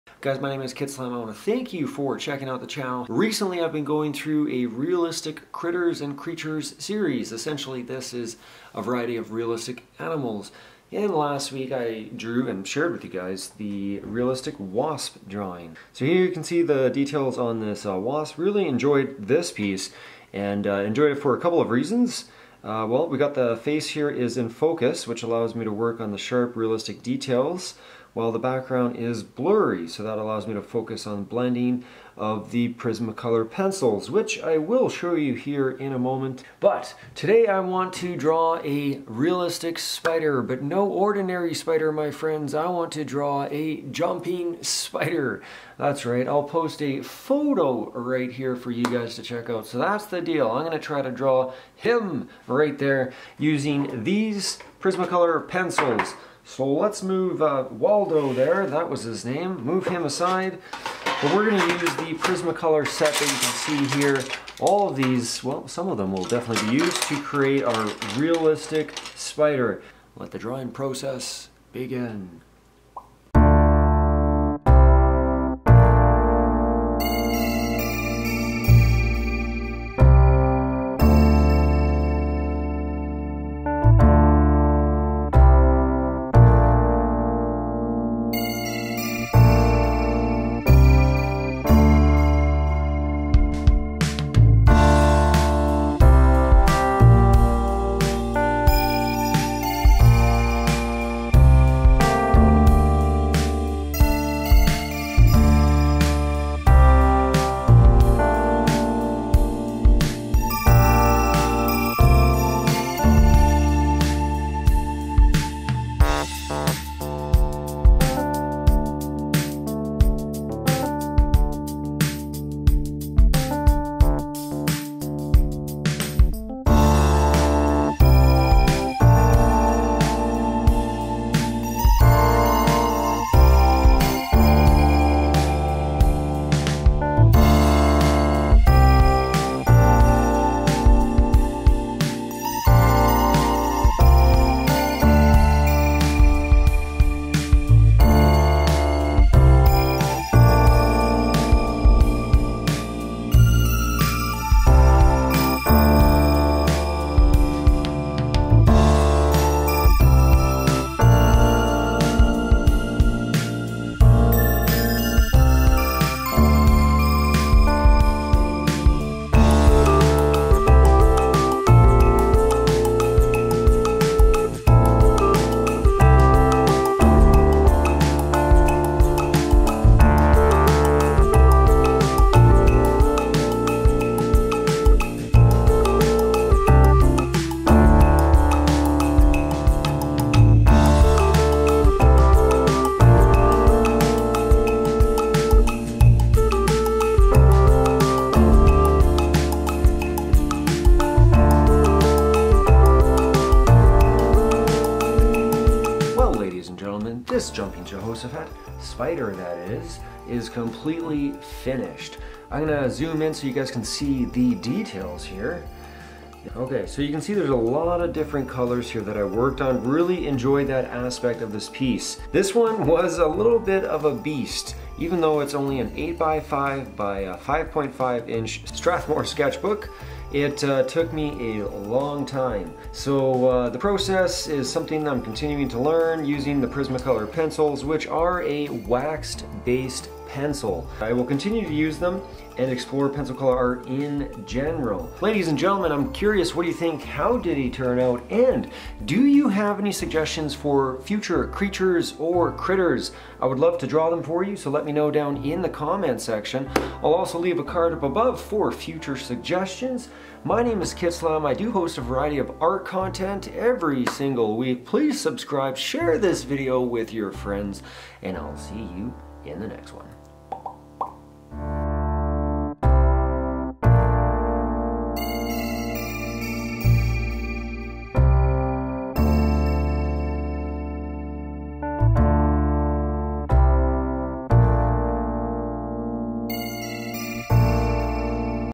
This REALISTIC Spider Drawing will make you JUMP (most likely)
It's my realistic drawing of a Jumping Spider, the same critter that inspired Lucas the Spider. This detailed spider drawing was realized with Prismacolor pencils and took over 30 hrs to complete. Whatcha think of our him, how'd he turn out? And share your suggestions for future Creatures or Critters in the comments below.